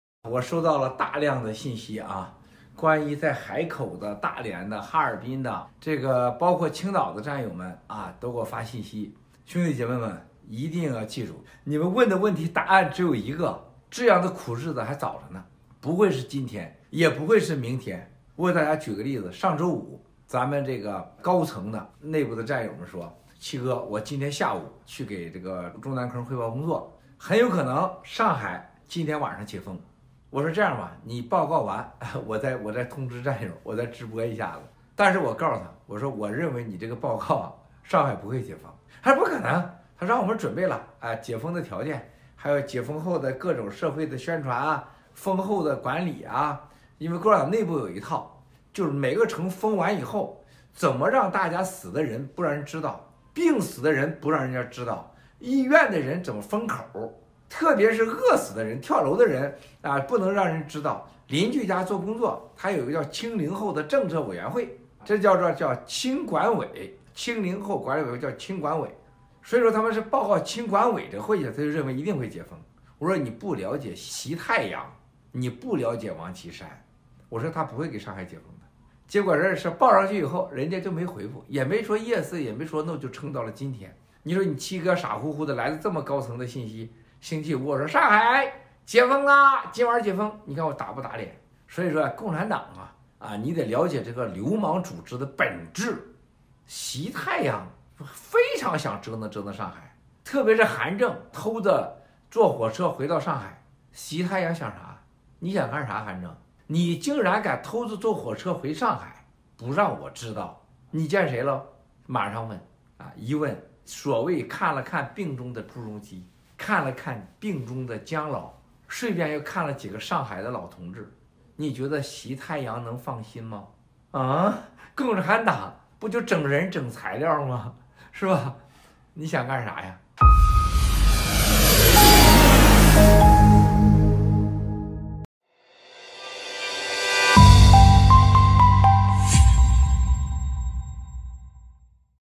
4/18/2022 文贵盖特：CCP高层的内部战友预计周五上海解封，却没有发生；只有七哥了解中共这个流氓组织的本质；习太阳想折腾上海，韩正偷坐火车回上海看望江朱，引习警觉
4/18/2022 Miles Guo’s GETTR: The removal of the restrictions in Shanghai on last Friday predicted by the insiders at the top level of the CCP did not happen. Only I understand the rogue nature of the CCP. “Xi the Sun” simply wants to make the people in Shanghai suffer, and Xi was alerted after he knew Han Zheng had secretly gone back to Shanghai by train.